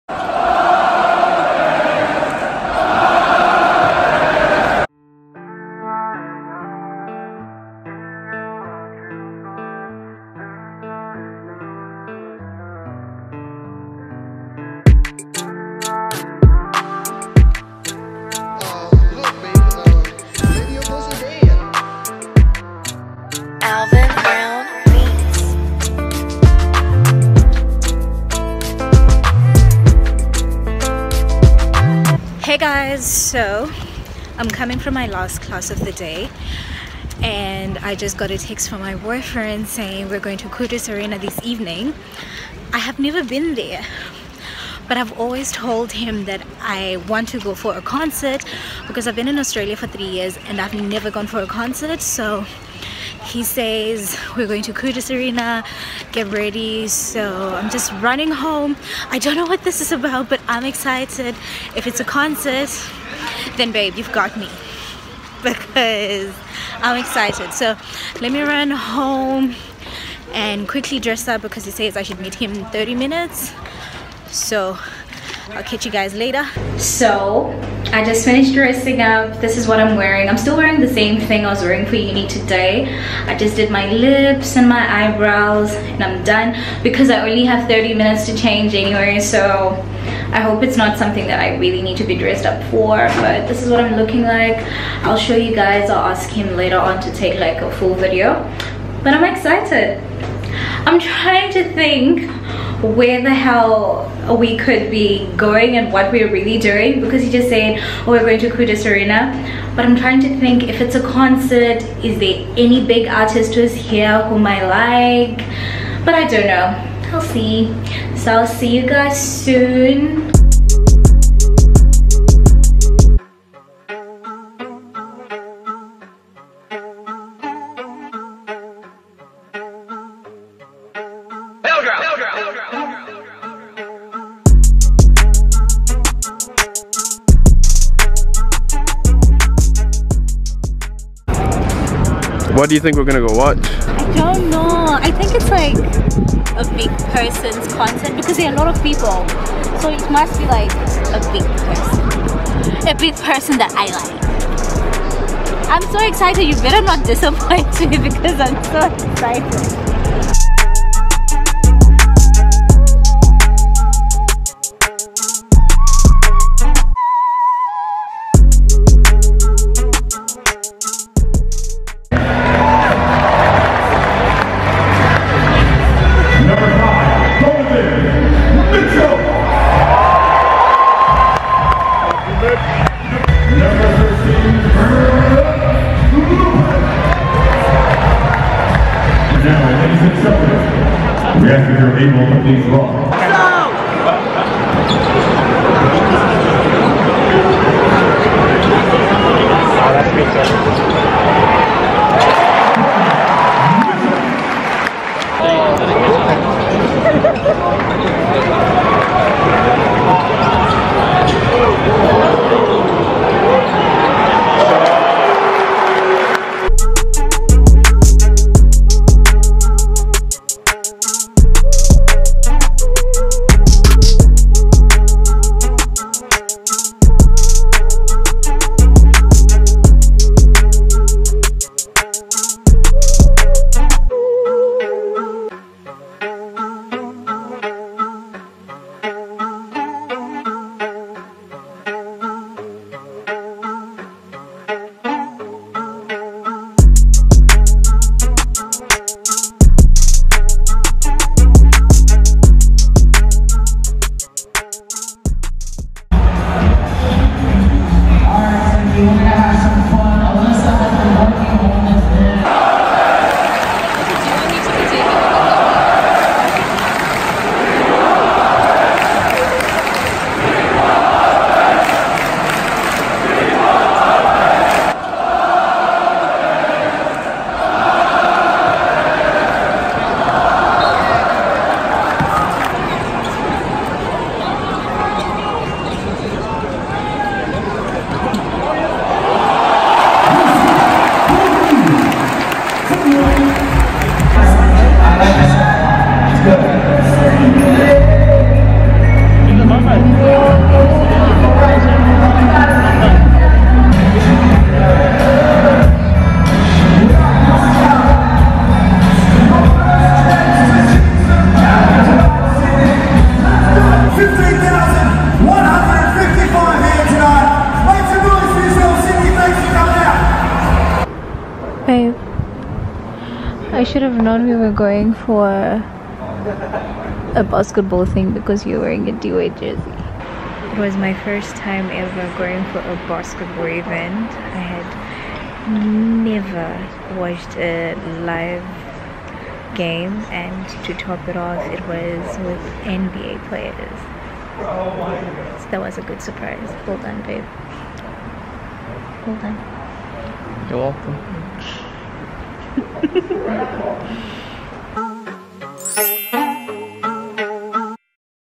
Australia loves Brook Lopez: Failed Surprise date night!
Hey my loves 💕. I had the opportunity to watch the USA 🇺🇸 vs Canada 🇨🇦Basketball game and I am sharing it with you guys. I really thought I was being taken for a surprise concert 🤦🏾‍♀️ but 😂 I enjoyed it still.

Artist: Alvin Brown Beats

Artist: Pilgrim Beats (prod.pilgrim)

KSD